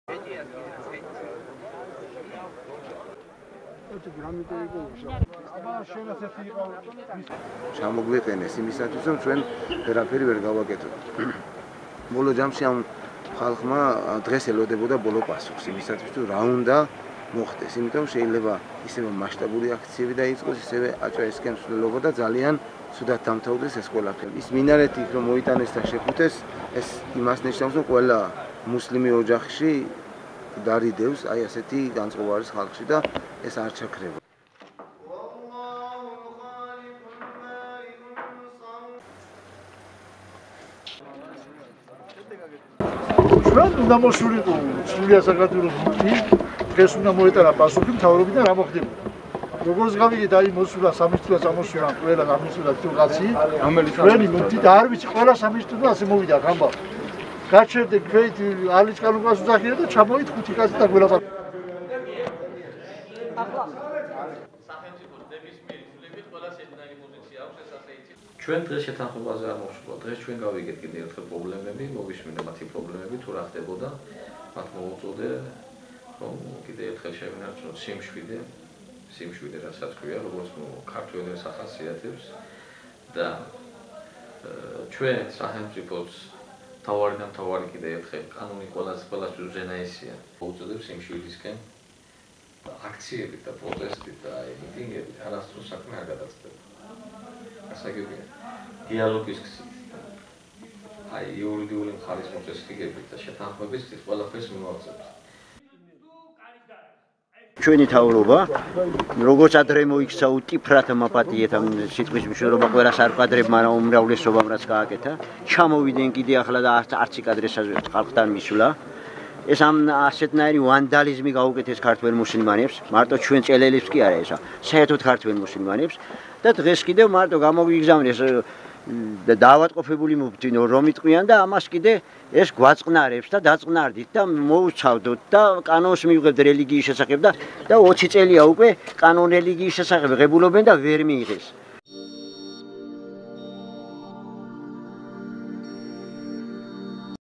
რა ხდებოდა ჭელაში 20 სექტემბერს [Video]
რატომ გაუცრუვდა იმედი ადიგენის მუსლიმურ თემს სრულიად საქართველოს მუსლიმთა სამმართველოს მუფთის ჯემალ პაქსაძის ჭელაში სტუმრობის შემდეგ.